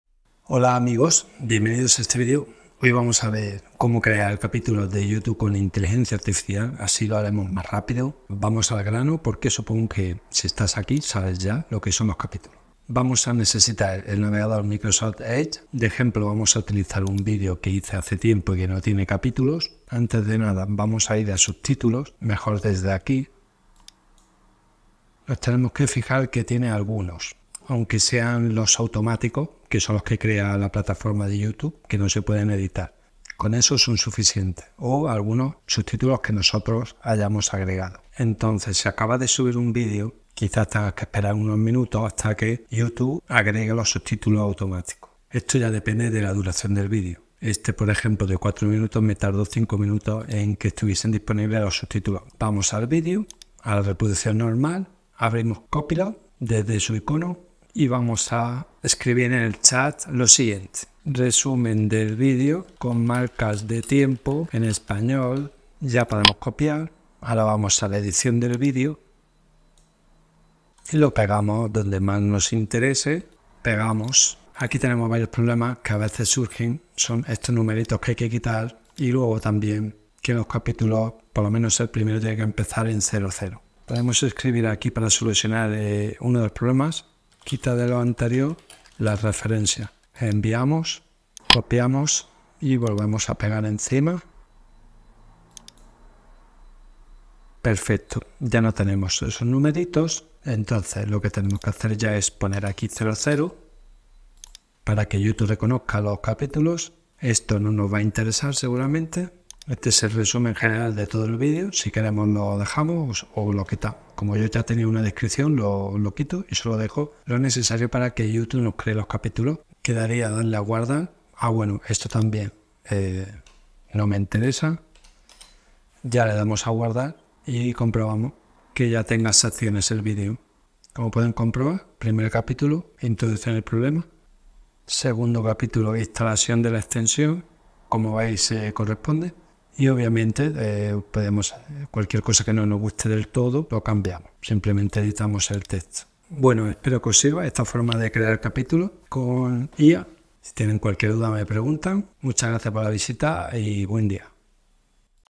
🤖 Capitulos Automaticos Con Ai Para YouTube 🆓 Create YouTube time tags with Copilot 🔱 2024
💻Este video muestra cómo crear capítulos en YouTube usando inteligenciaartificial y el navegador Microsoft Edge. El proceso incluye usar subtítulos automáticos de YouTube, generar un resumen con marcas de tiempo (Timetags) y editar el texto para que YouTube reconozca los capítulos.

[00:00:00] Introducción Usar MicrosoftEdge.
[00:00:21] Necesario que el vídeo tenga subtítulos automáticos o agregados manualmente.
[00:00:54] Generar resumen usando el chat de Copilot.
[00:01:36] Corregir texto y verificar capitulos.

Espero que esta información te sea útil. ¿Hay algo más en lo que pueda ayudarte?